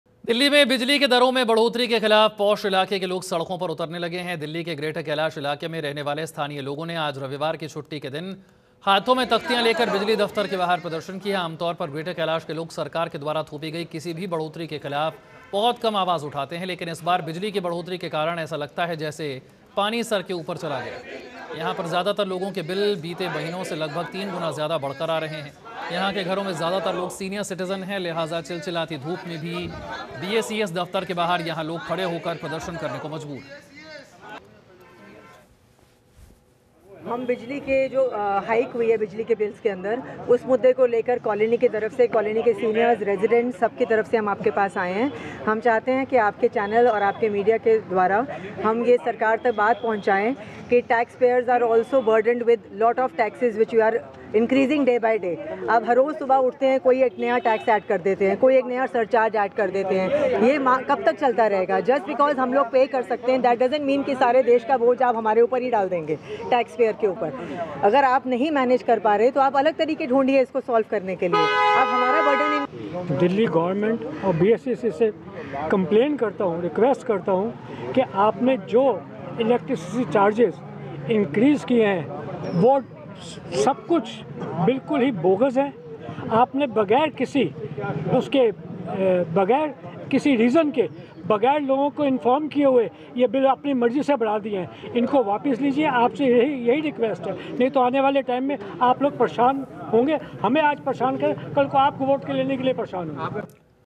बिजली दरों में बढ़ोतरी को लेकर विरोध, BISI दफ्तर के बाहर प्रदर्शन !
About Video